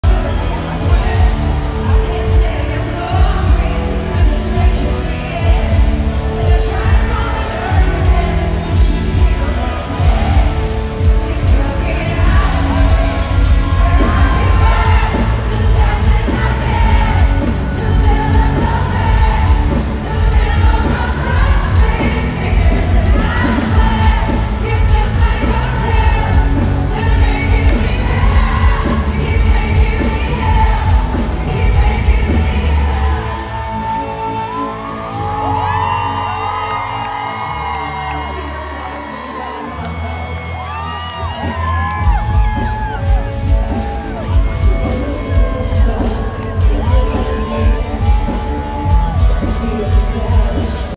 P!nk concert at the Rocket Mortgage Fieldhouse in Cleveland, Ohio.
Seats were about 10 rows to the front of the stage, 5 rows to the right of the walk-out front area.